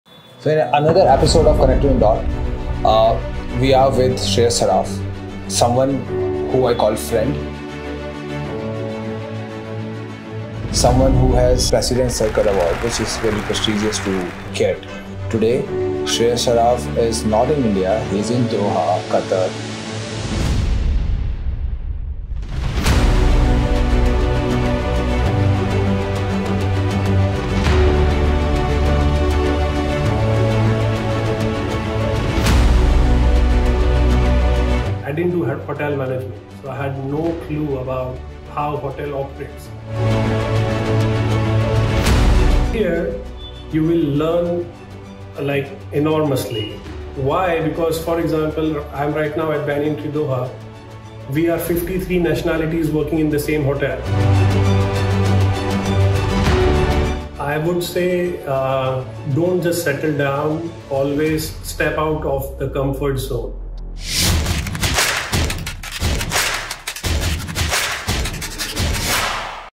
R-own presents Connecting the dots Ft. Shreyas Saraf
Join us for an inspiring conversation with hospitality expert Mr. Shreyas Saraf, hosted by his friend Mr. Prashant Mishra. Discover Shreyas's journey from Assistant Director of Sales at Banyan Tree Hotels, Doha, to receiving the President Circle's Asia Pacific Award. Don't miss this engaging episode!